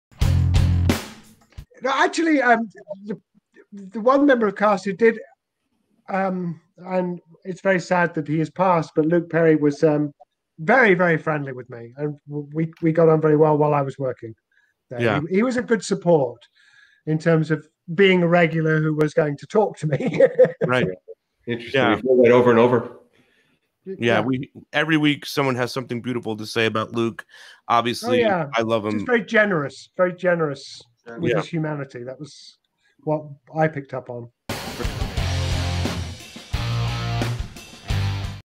Jason Carter Pays Tribute To Luke Perry
A beautiful sentiment by Jason Carter on the incredible Luke Perry.